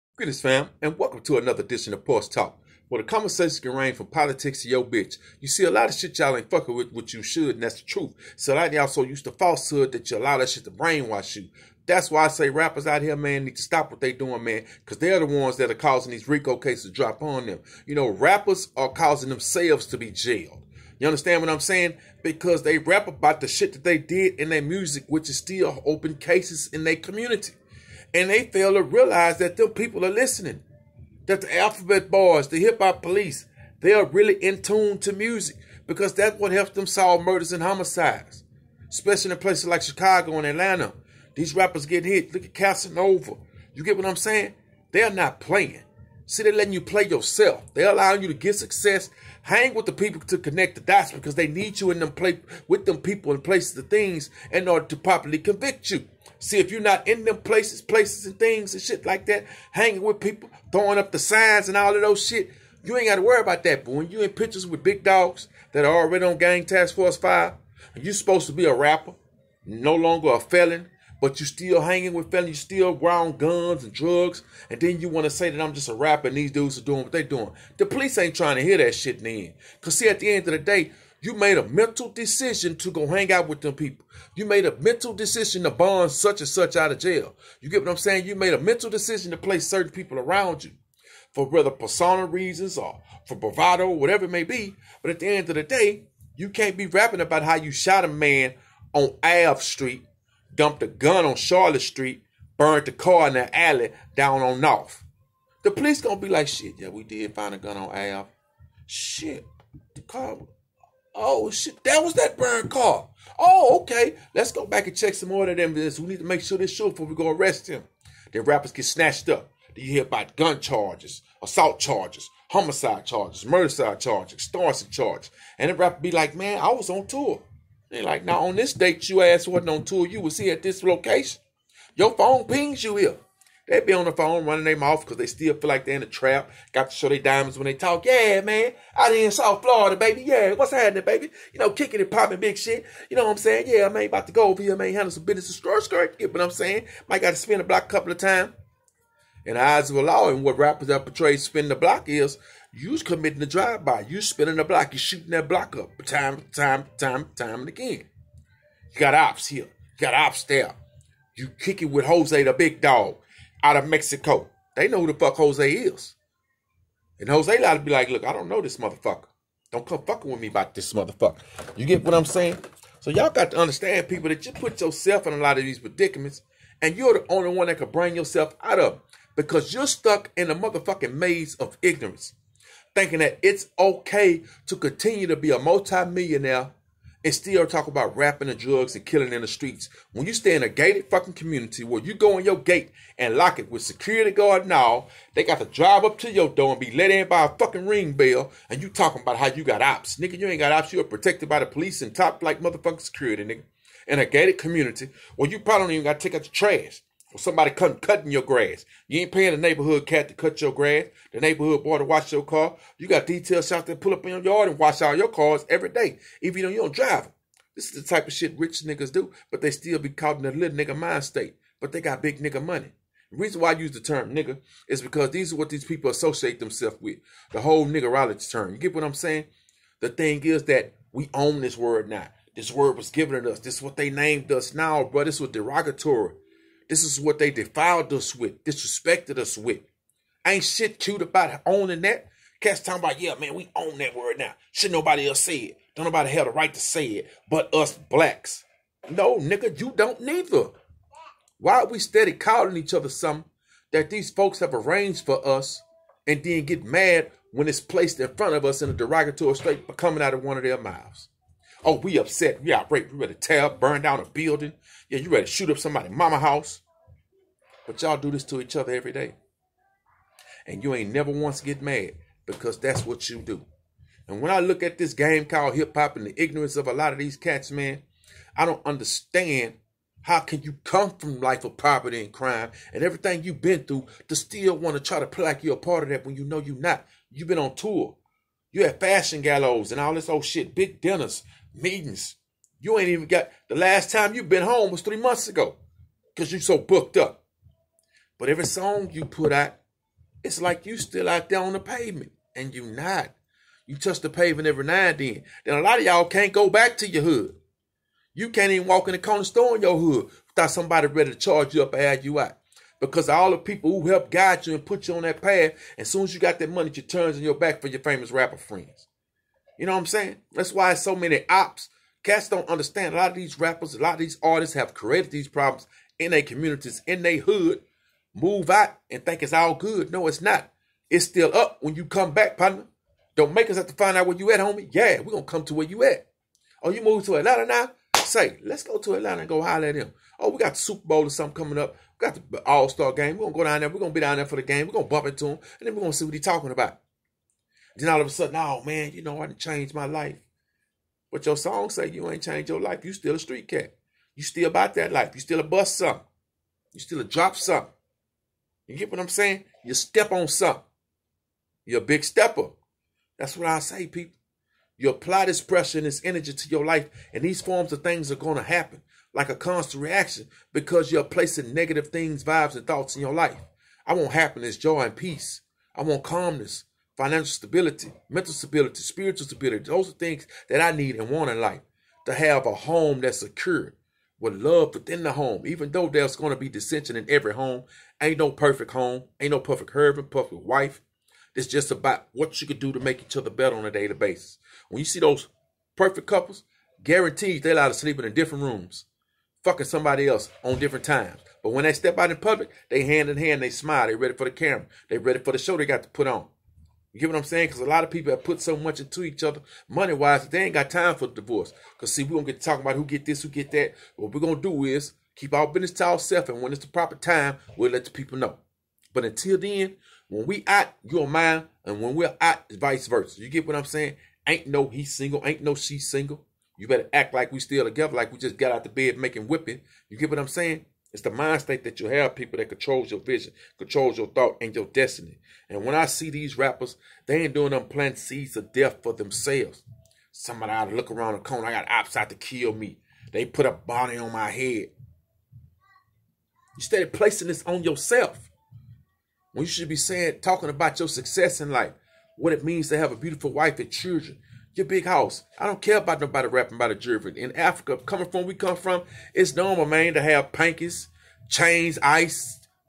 " Porch Talk " How Rappers are writing their own indictments thru their music
Rappers cry wolf all the time and expect people to go bat for them when they tell in themselves thru their songs.